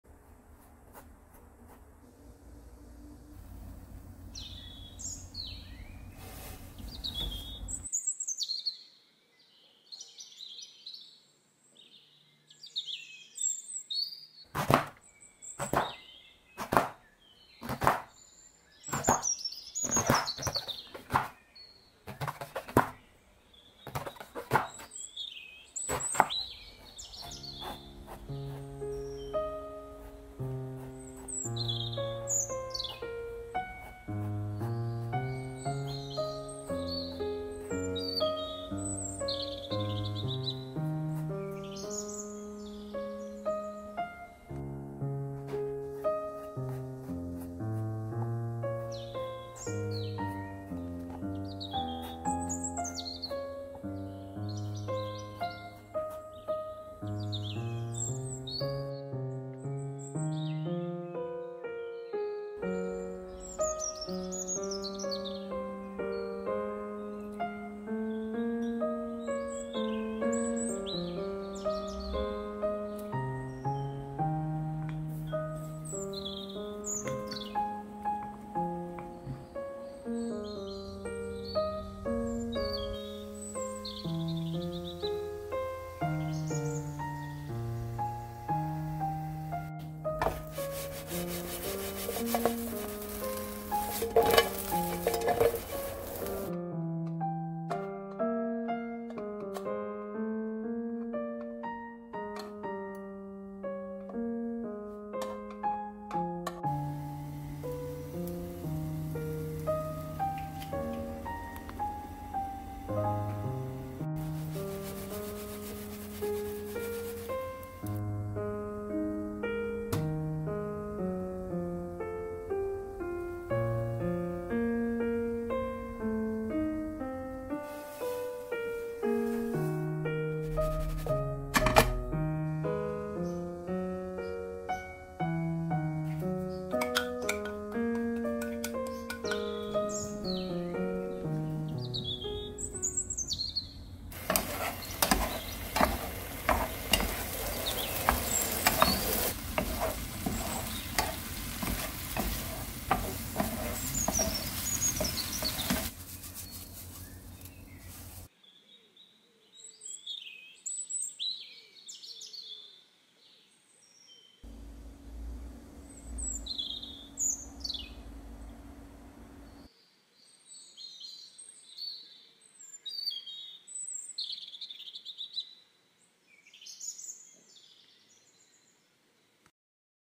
가지반찬 가지구이볶음 가지요리맛있게하는법
가지요리
가지구이볶음

가지 3개
진간장 2
참치액젓 1
굴소스1
올리고당 1
다진마늘 1
청홍고추 대파 조금
참기름 1
통깨 1